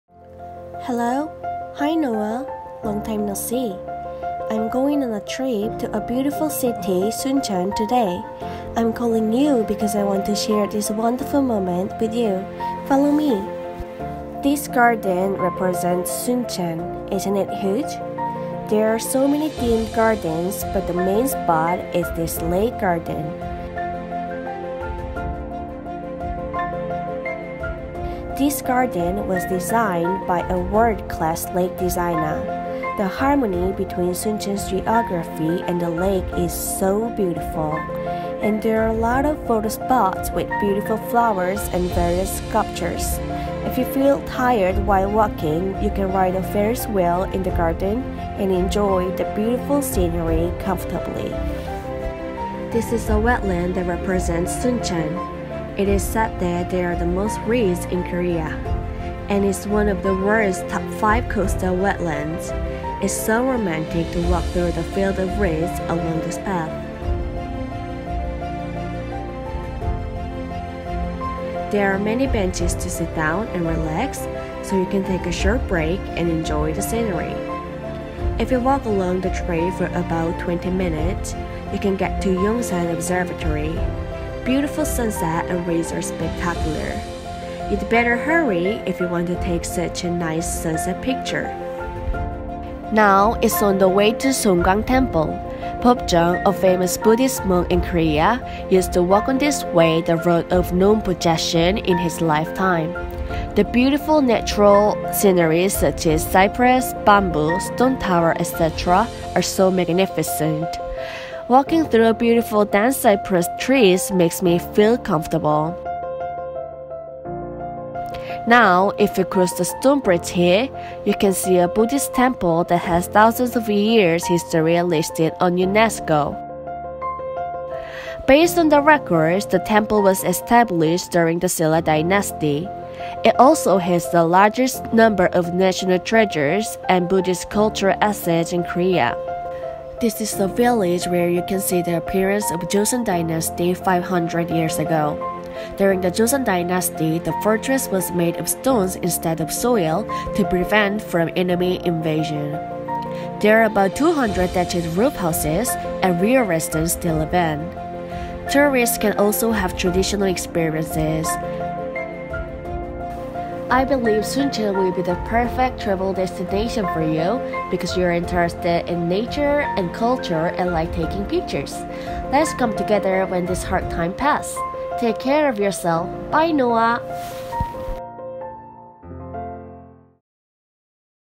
Best places to visit in Suncheon, Korea
Suncheon is a beautiful city in Korea. It's the biggest colony of reeds in Korea and one of the world's top 5 wetlands.

Find out more attractions in Suncheon through the video!

1. Suncheonman Bay Wetland Reserve
2. Yongsan Observatory
3. Songgwangsa Temple
4. Naganeupseong Folk Village